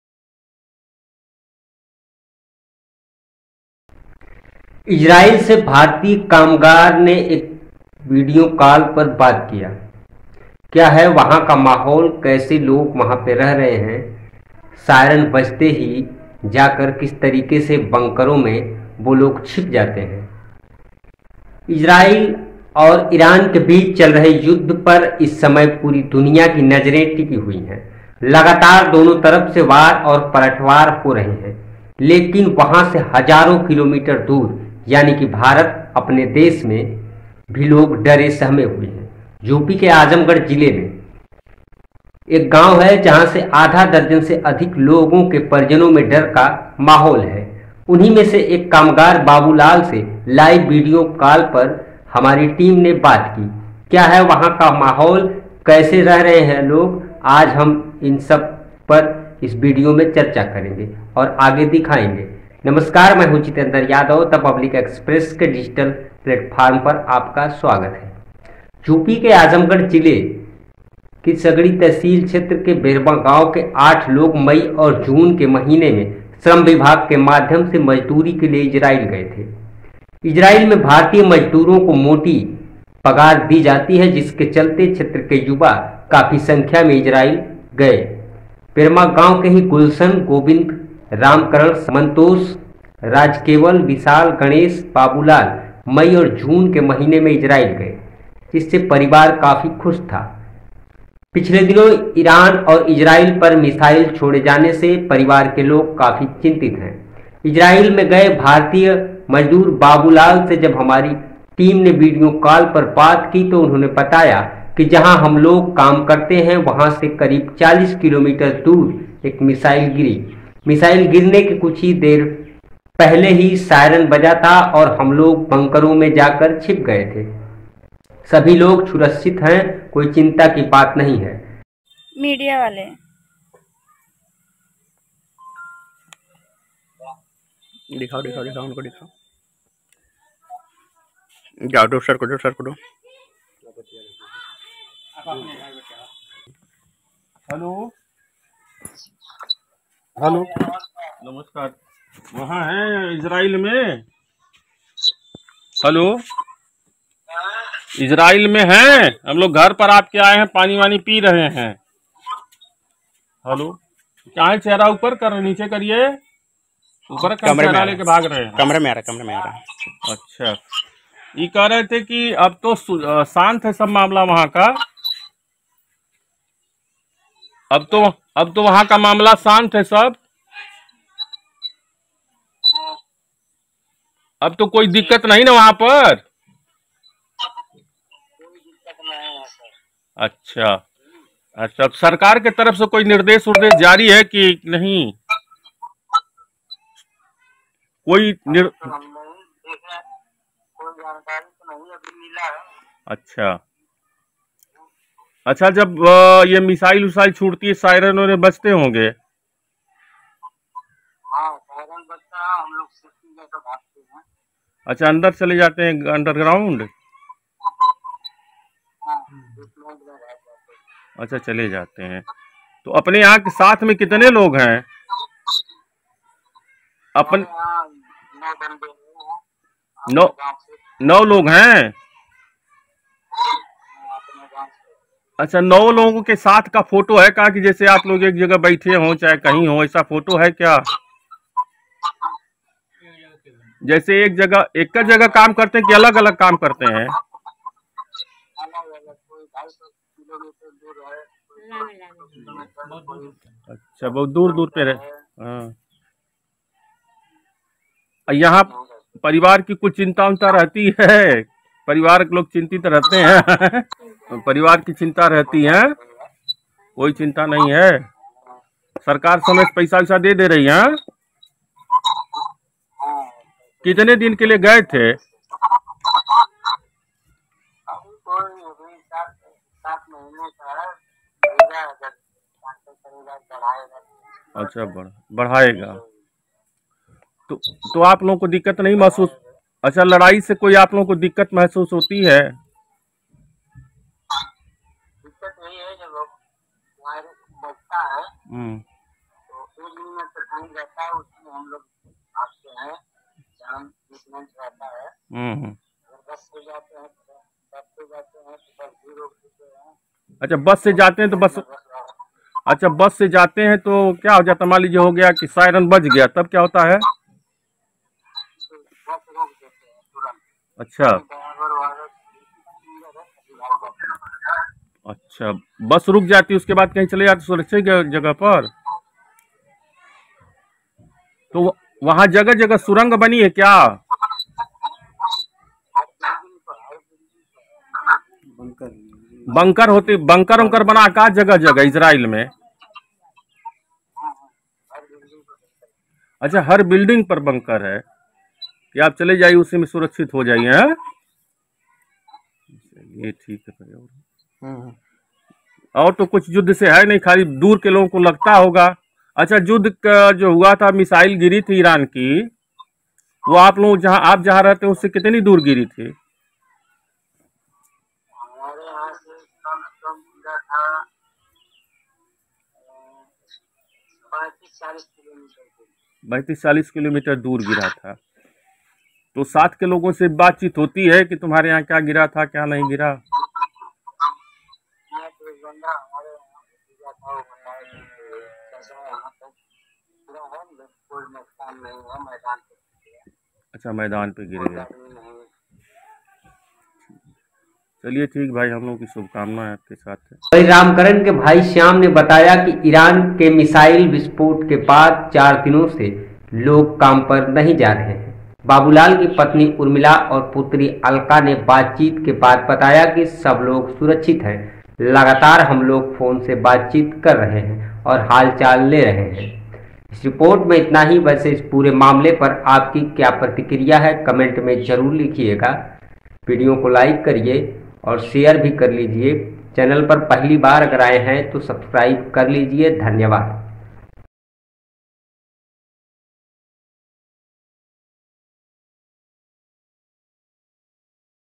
Iran vs Israel War | जब मिसाइल गिरती है तो....भारतीय श्रमिक ने वीडियो कॉल पर बताया !
इजरायल में भारतीय श्रमिक, indian worker in Israel, Azamgarh news, Azamgarh worker in Israel,